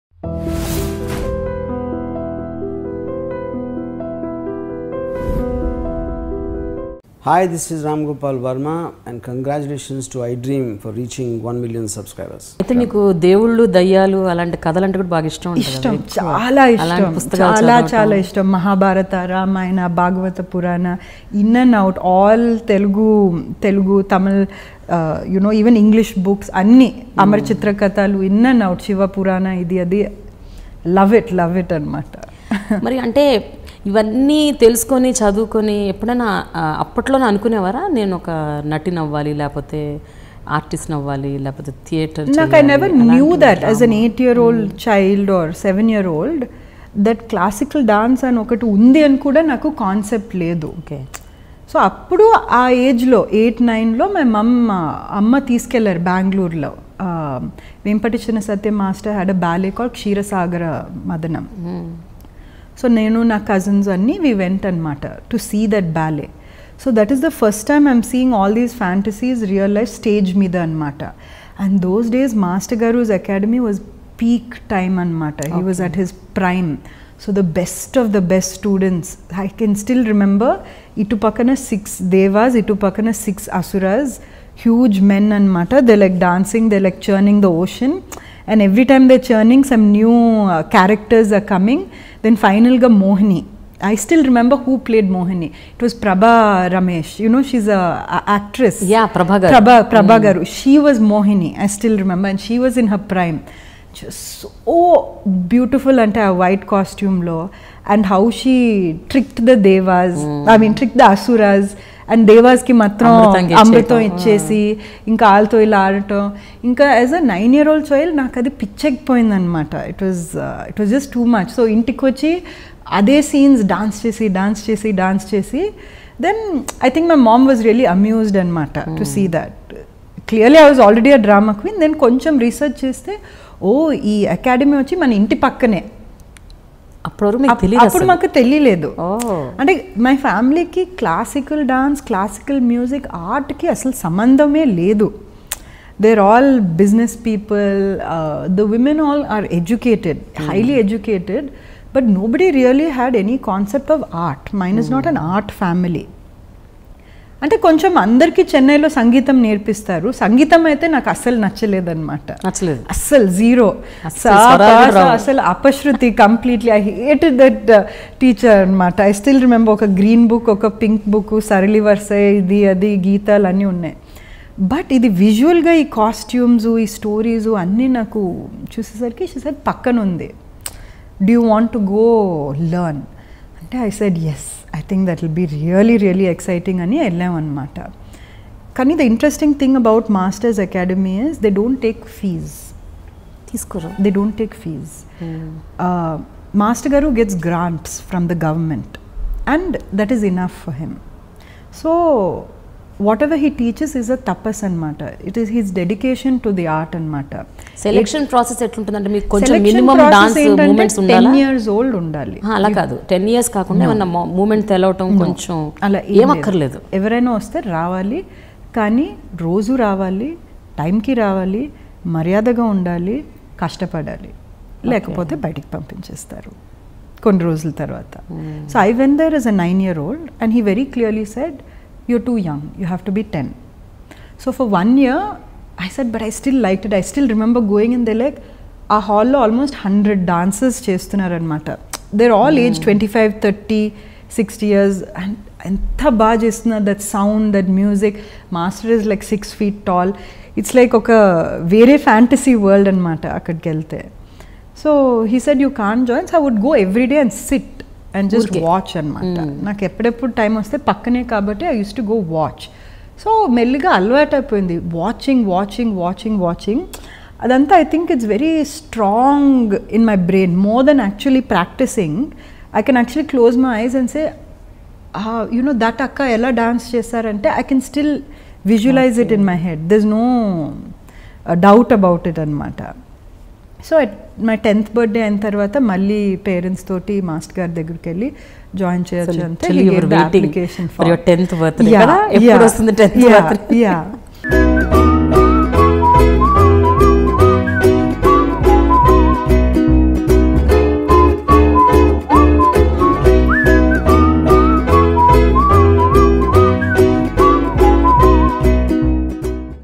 That Was The Most Memorable Moment Of My Life - Sandhya Raju || Dialogue With Prema
👉 Episode 1 Sandhya Raju is a Kuchipudi professional who represents the next generation of Indian classical dancers. With 12 years of training under Gurus Padma Bhushan Vempati Chinna Satyam and Kishore Mosalikanti and 8 years performing experience, Sandhya has been performing several solo and group recitals across India and abroad. Apart from teaching at Nishrinkala, Sandhya also owns and manages Barefoot Dance Company in Hyderabad.

She was born in Chennai in 1987 and completed her formal studies there. She moved to Hyderabad in 2006 after her marriage to Mr. Rama Raju and began teaching dance in June 2007. She is the Artistic Director of Nishrinkala Dance Academy, Hyderabad.